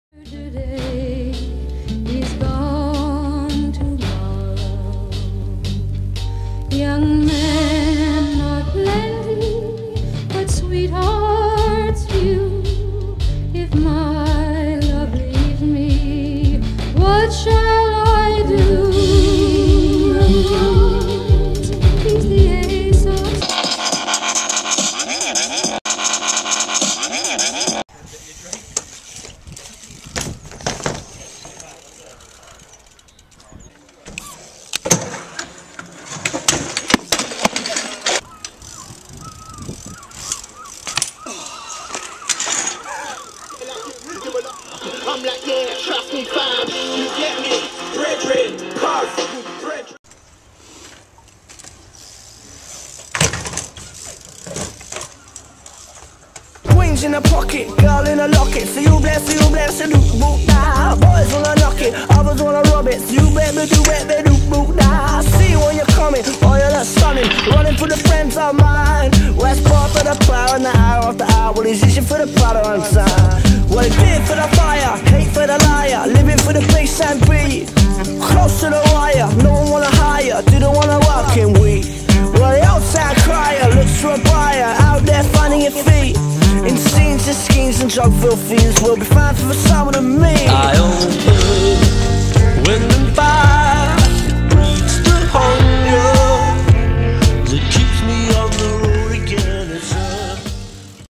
- Areyasure || Sessions -
Filmed a session in exmouth park , with me luan and jordan , sick end trick from jordan !!